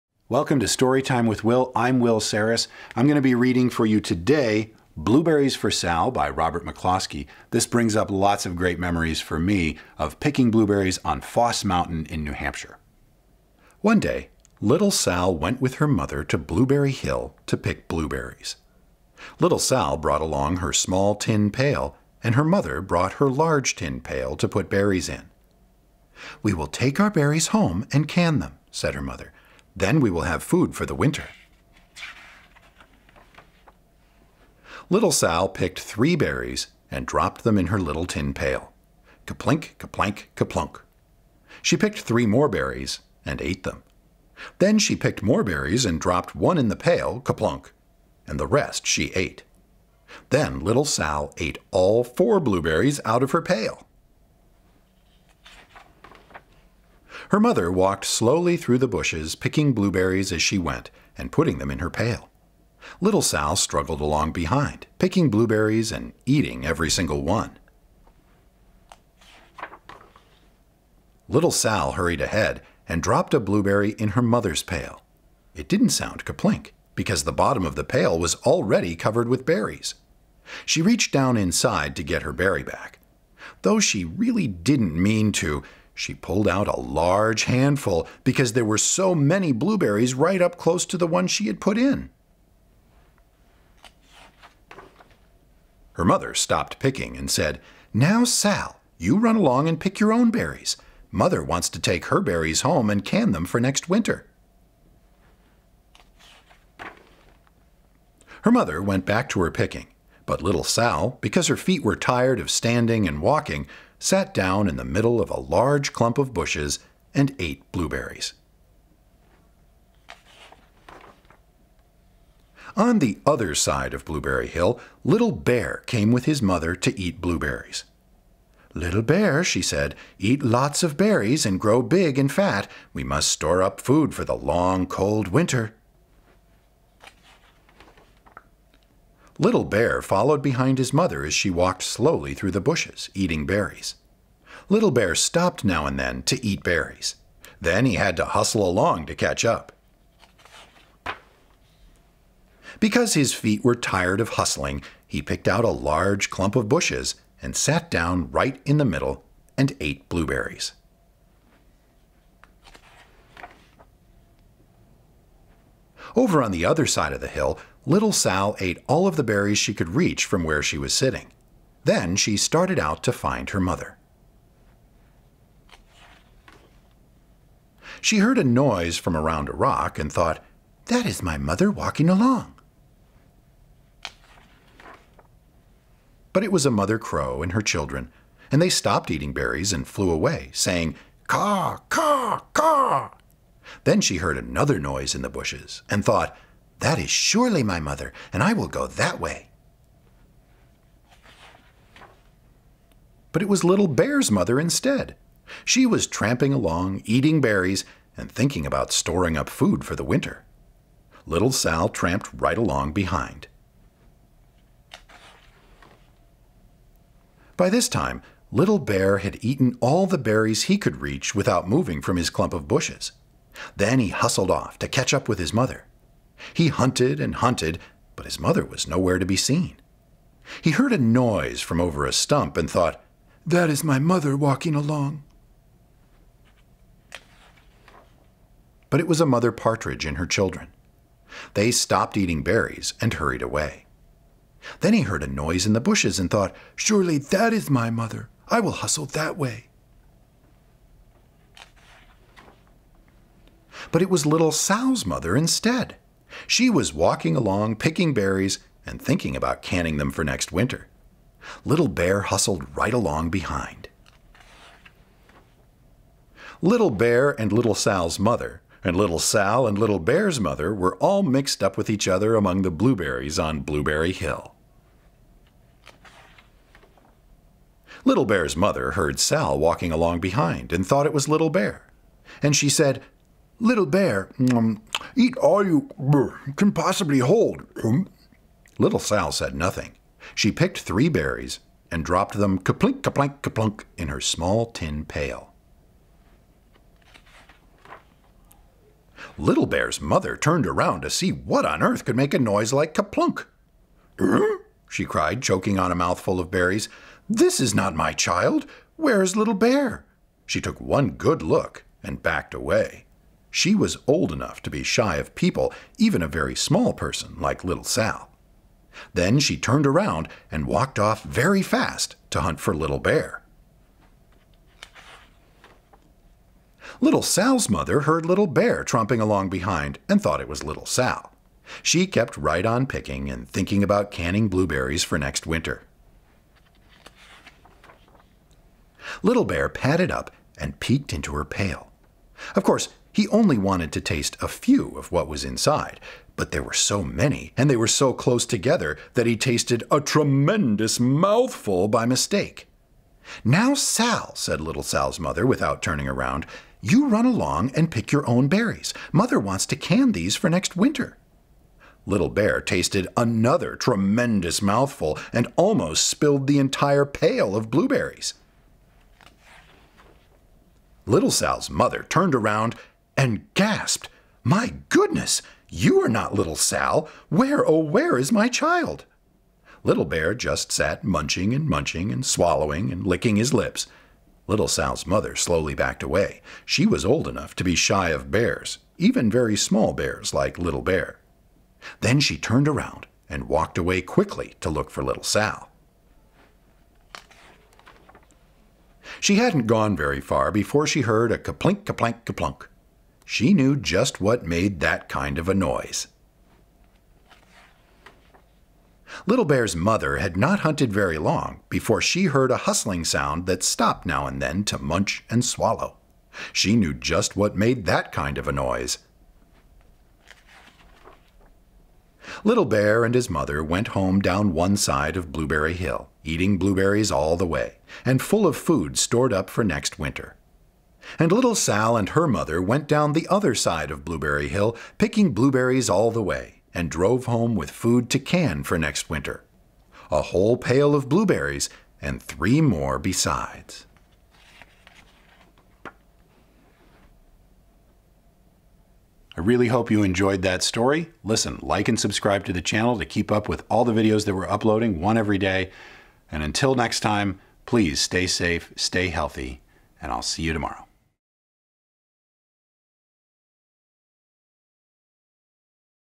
Blueberries for Sal by Robert McCloskey (Read by Will Sarris)
This is a classic story that appeals to kids of all ages with its humor. My family and I used to go to a mountain in New Hampshire with a large wild blueberry patch, and it looked a lot like the illustrations in this book!

For more information about Will's voiceover or video production services, head